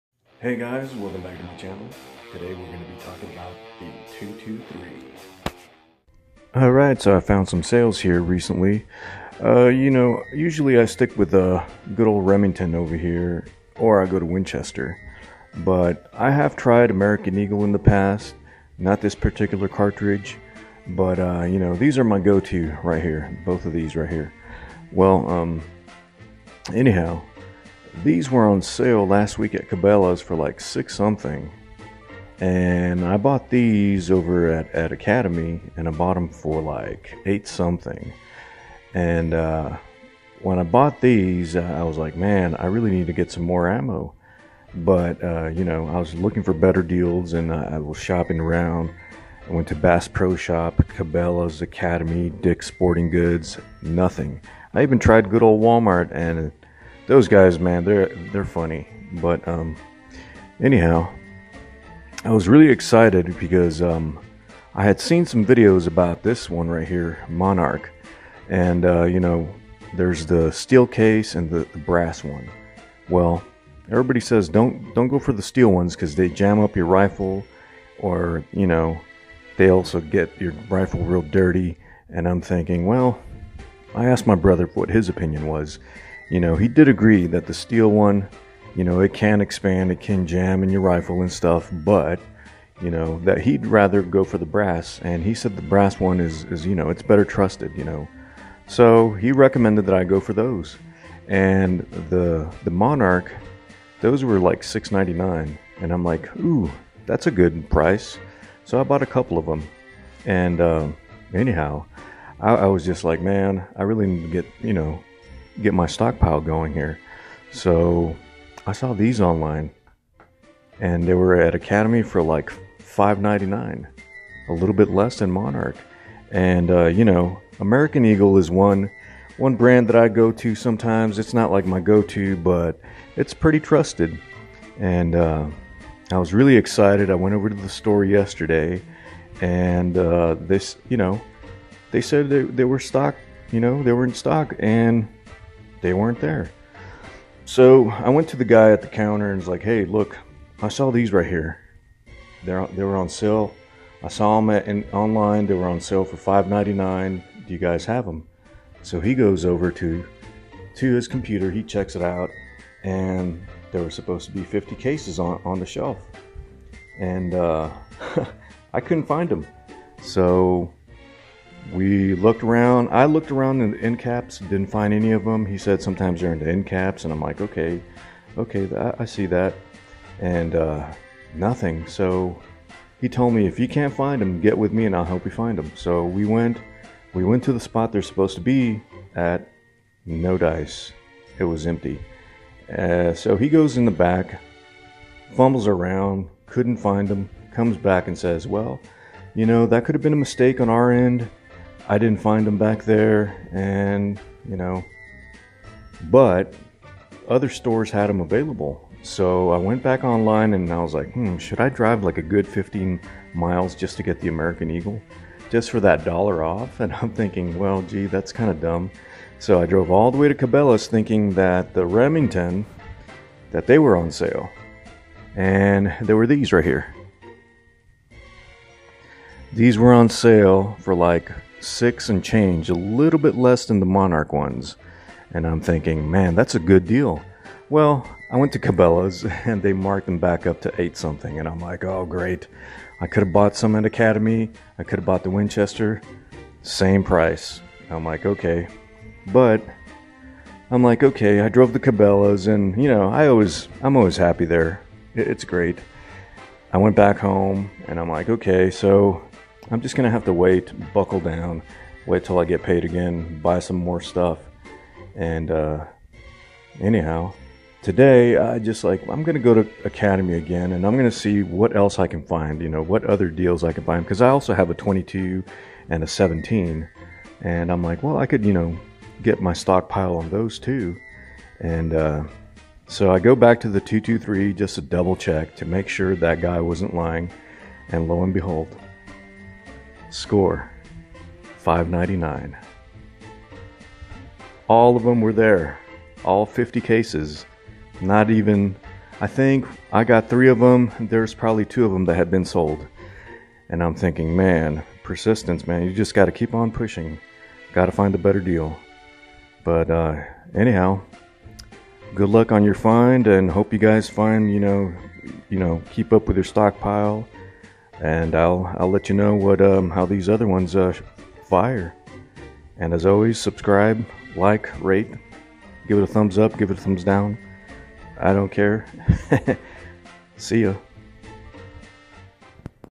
223 Ammo score! $5.99 20ct@ Academy
Monarch 223
55grain Brass Case
@ Academy $6.99
American Eagle 223
55grain Brass Case
@ Academy $5.99